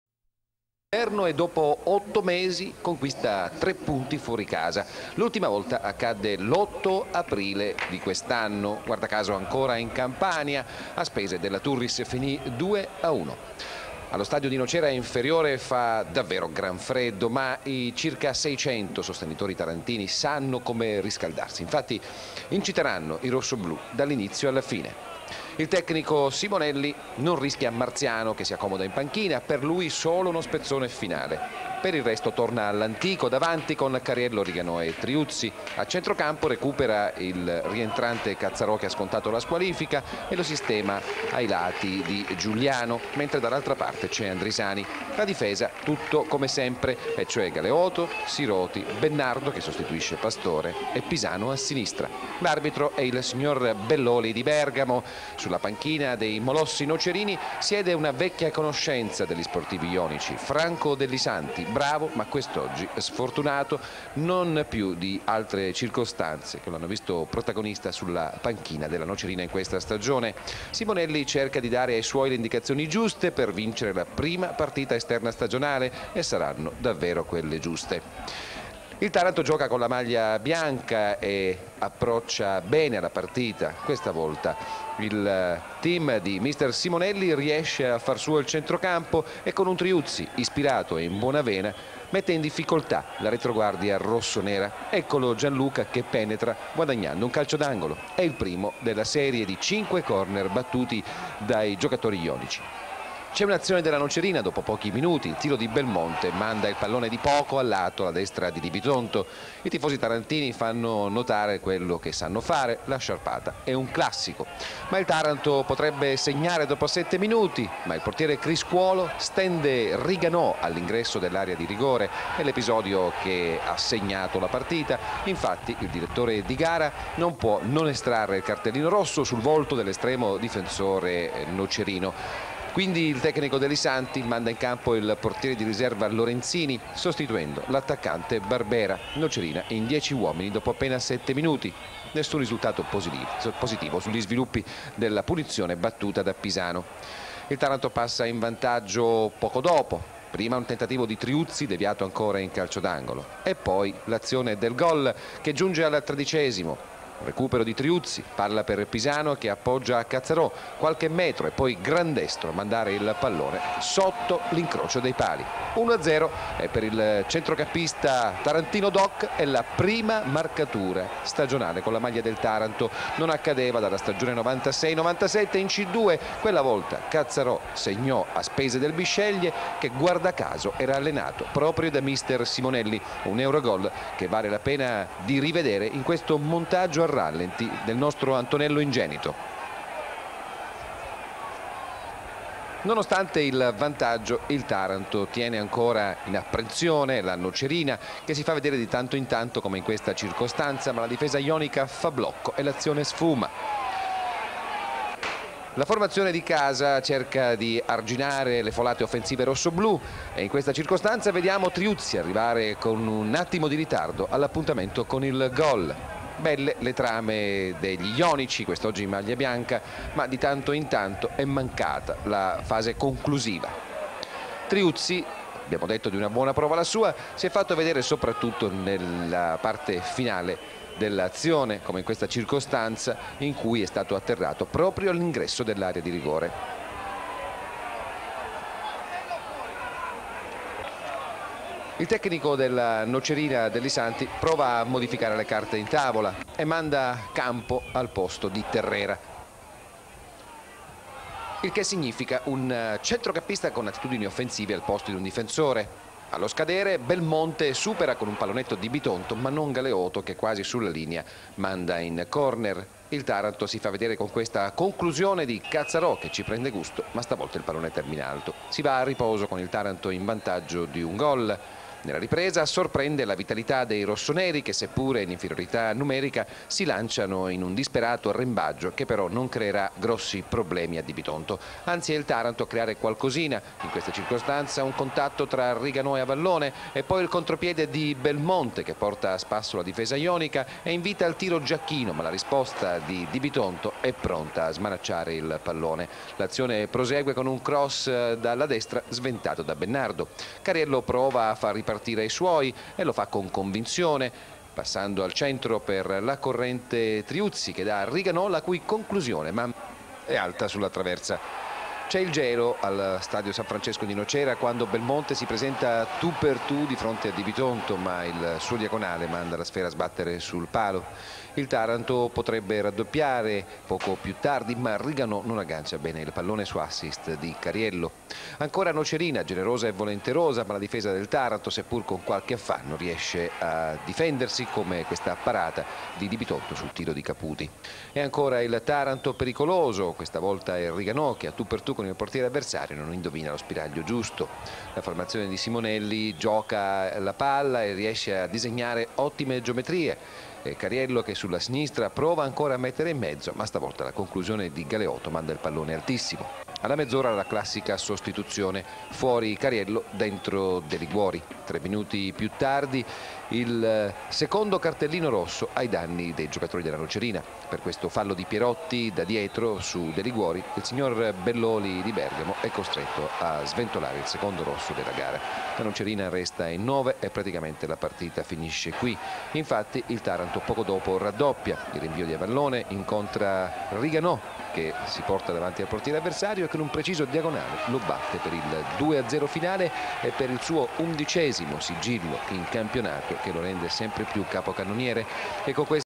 Nocerina Taranto 0 2 Campionato C1B 2001 2002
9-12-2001 - QUINDICESIMA GIORNATA  NOCERINA-TARANTO 0-2 -Nocerina: Criscuolo; Terriera (30' Campo), S. Avallone, Langella, Pierotti, Nocera (82' Turchetti), Lillo, Caputi, Giacchino, Barbera (8' Lorenzini), Belmonte. A disposizione: Ciardiello, De Rosa, Alfano e Del Prete. Allenatore: Dellisanti.
Taranto: Di Bitonto; Galeoto, Pisano, Giugliano, Siroti, Bennardo, Cazzarò, Andrisani (84' Marziano), Riganò, Triuzzi, Cariello (80' Deliguori). A disposizione: Signorile, Vitali, Pizzolla, Ferraioli e Di Nardo. Allenatore: Simonelli.
Arbitro: Belloli di Bergamo.
Marcatori: 13' Cazzarò, 84' Riganò.
Note: ammoniti Langella, Lillo, Andrisani e Cazzarò; espulsi Criscuolo e Pierotti.